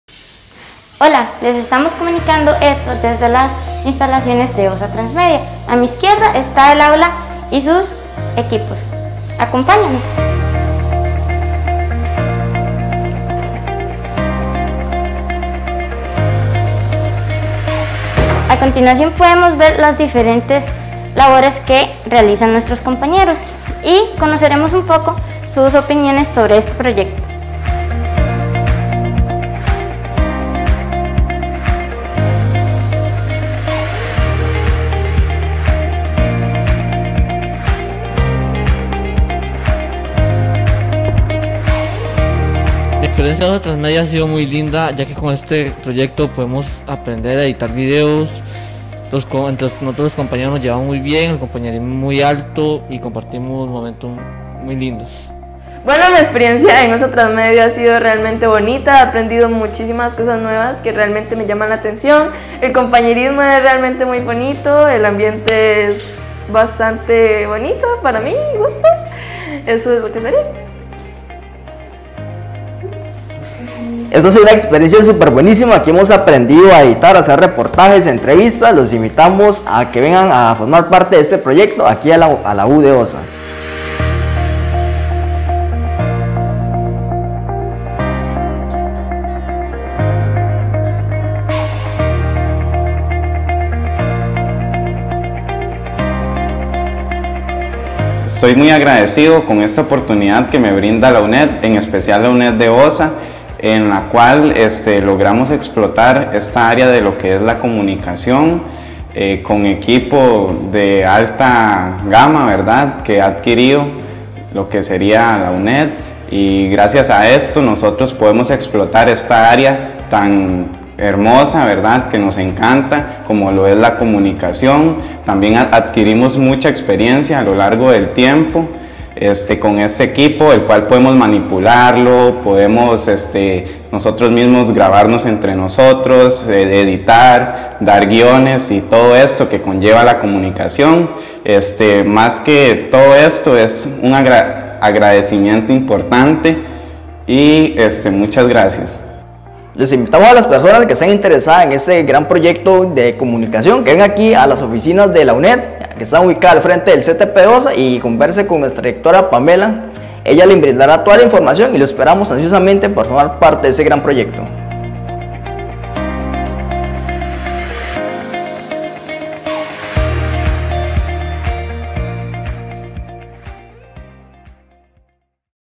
UNED SEDE OSA TIENE IMPORTE PROYECTO AUDIO VISUAL LLAMADO OSA TRANSMEDIA
Chicos del proyecto de la Uned sede Osa, presentan su proyecto y dan su opinión sobre la experiencia